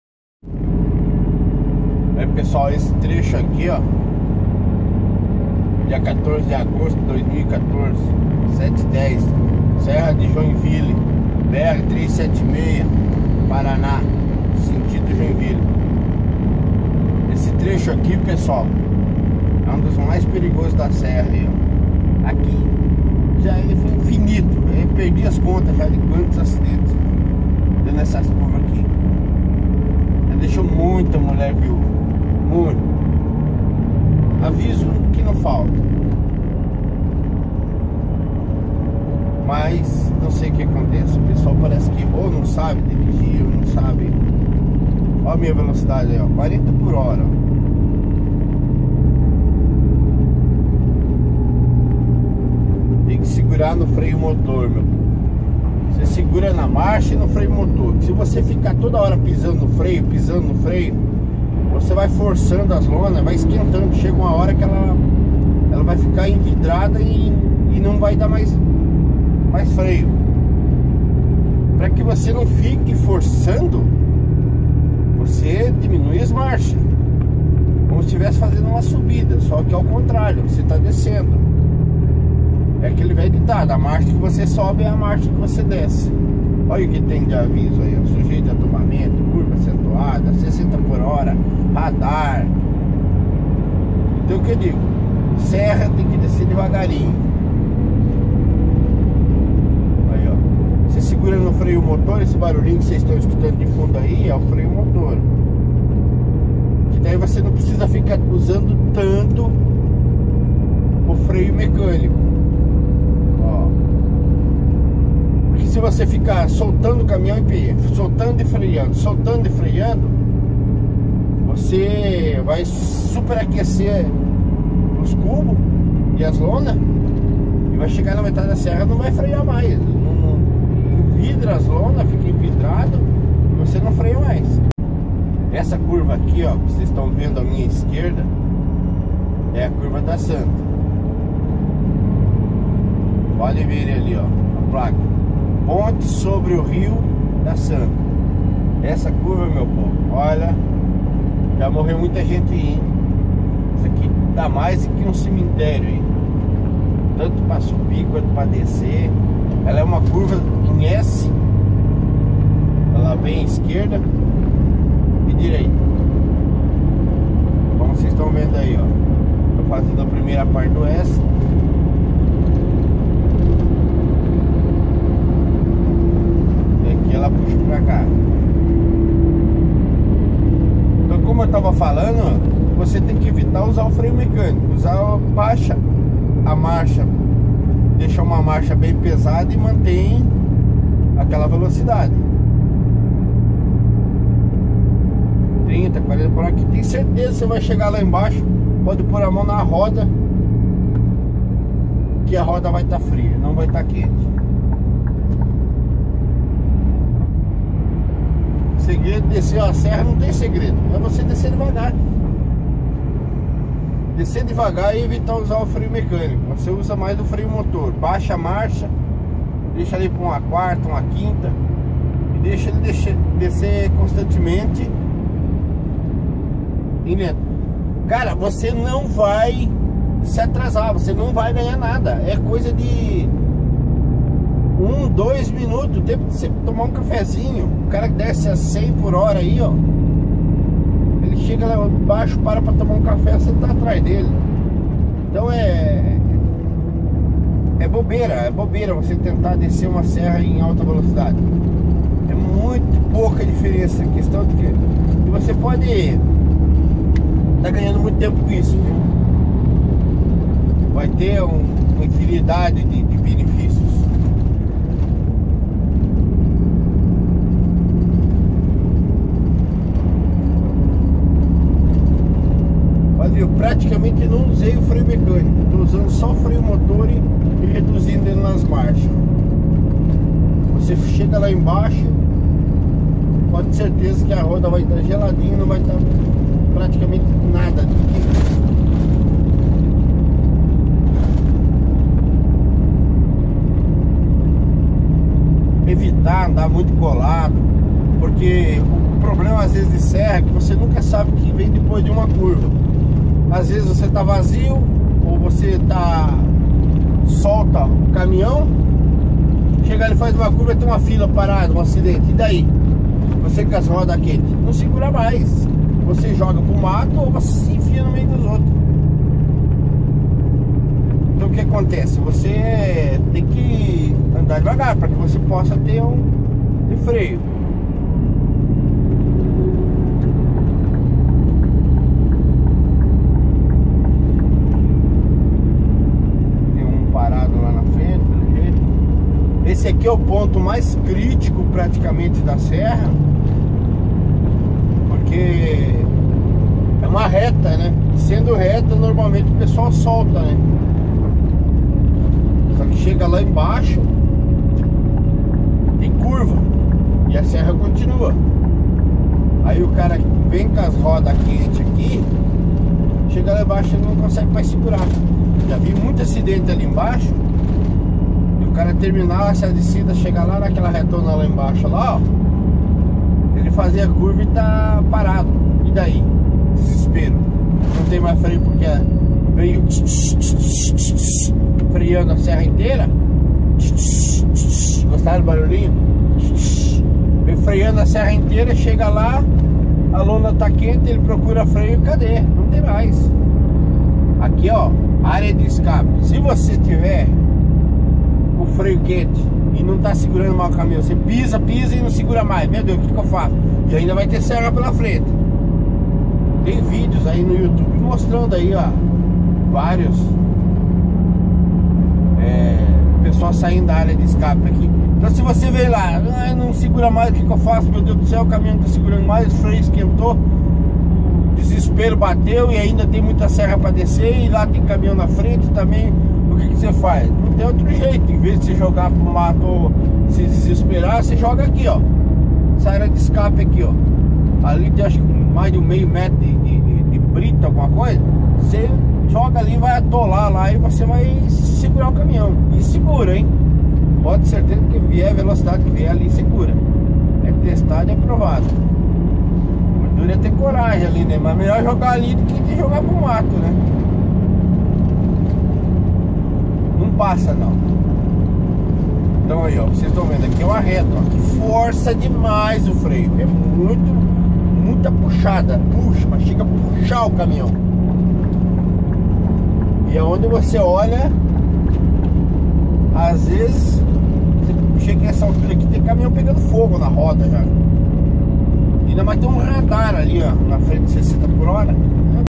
Descendo a serra de Joinville
Descer uma serra requer paciência... se você estiver apressado eu lhe digo que você está na profissão errada. A vida vale mais que 1 ou dois minutos.